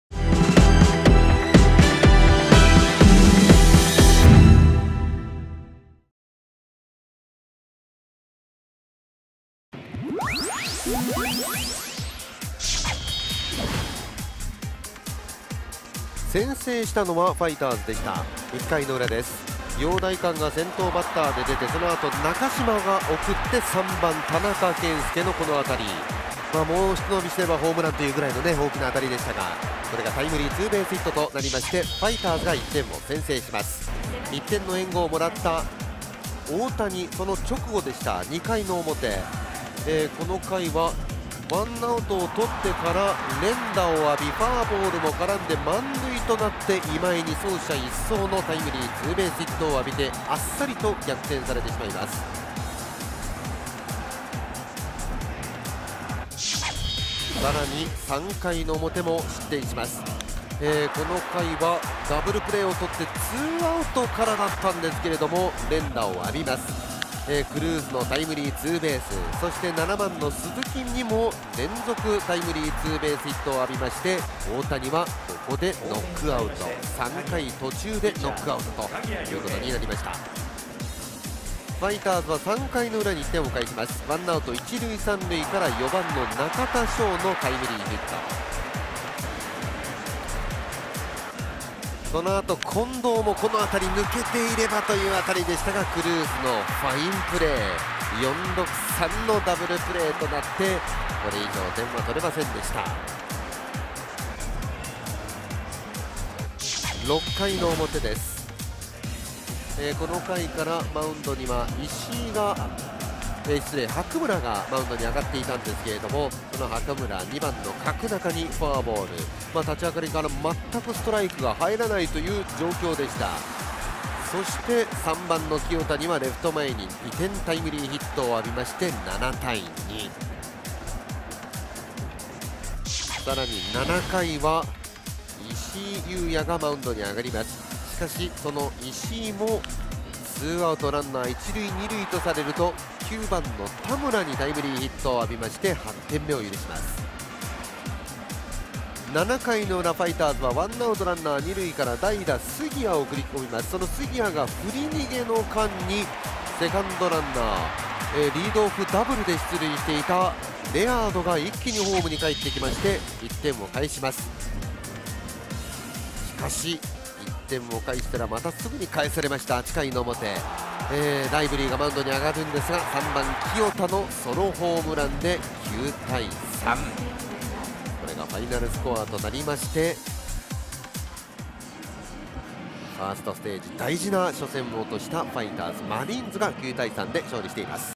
【大谷翔平まさかの敗戦…】千葉ロッテ、3位からの”下克上”なるか？
千葉ロッテが先勝し、ファイナルステージ進出にあと1勝と王手。先発･石川がCS初登板で5回2失点と初勝利。2回に今江敏晃の逆転タイムリーで2点リードすると、3回以降も順調に加点。8回には清田育宏がCS通算3本目となる1号ソロホームランを放った。北海道日本ハムは、先発・大谷翔平が2回2/3を5失点でCS初黒星。淺間は高卒新人では、パ・リーグ史上初、11年東京ヤクルト･山田哲人以来2人目となるCSスタメン出場を果たした。(2015年10月10日 北海道日本ハムファイターズ対千葉ロッテマリーンズ 試合ハイライト)